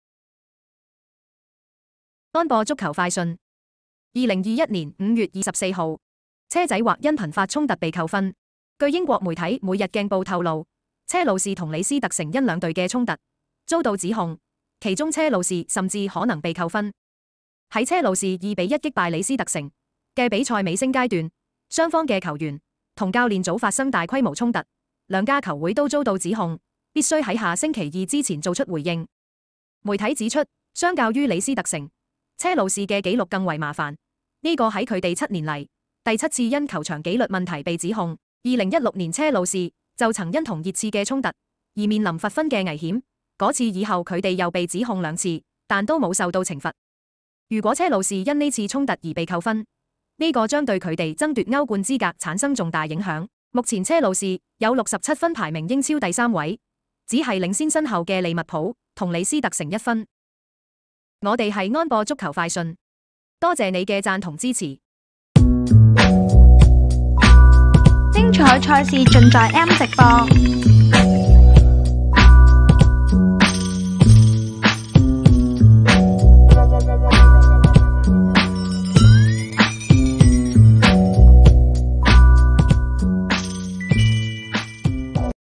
【英超- 花絮新聞】 車仔或因頻發衝突被扣分 2021-05-23
據英國媒體《 每日鏡報 》透露，車路士 和 李斯特城 因兩隊的衝突遭到指控，其中車路士甚至可能被扣除積分。

在車路士2比1擊敗李斯特城的比賽尾聲階段，雙方的球員和教練組發生大規模衝突，兩家球會都遭到指控，必須在下週二之前做出回應。

媒體指出，相較於李斯特城，車路士的紀錄更為糟糕，這是他們7年來第7次因球場紀律問題被指控，2016年車路士就曾因與熱刺的衝突而面臨罰分的危險，自那以後他們又被指控兩次，但都沒有受到懲罰。

如果車路士因這次衝突而被扣分，這將對他們爭奪歐冠資格產生重大影響，目前車路士積67分排名英超第三位，僅領先身後的利物浦和李斯特城1分。

►單場賽事低至MOP$15, 立即下載M直播觀看#韓職 亞洲足協杯 澳洲職業籃球 NBA 精彩賽事